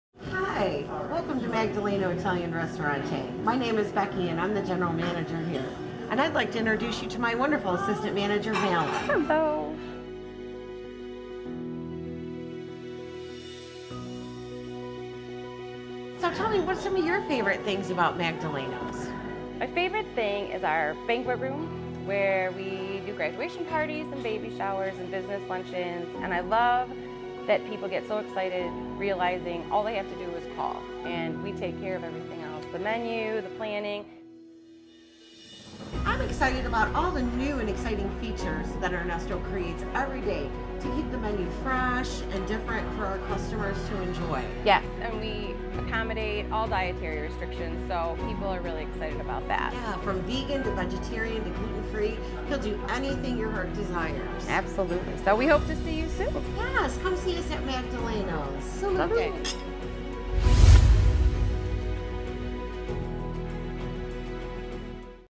Magdaleno's Downtown Wyandotte, MI.
Magdaleno's Downtown Wyandotte, MI. Tucked away just off the main drag of Biddle Avenue lies a veritable treasure trove of unique, fresh Italian food — better known as Magdaleno Italiano Ristorante.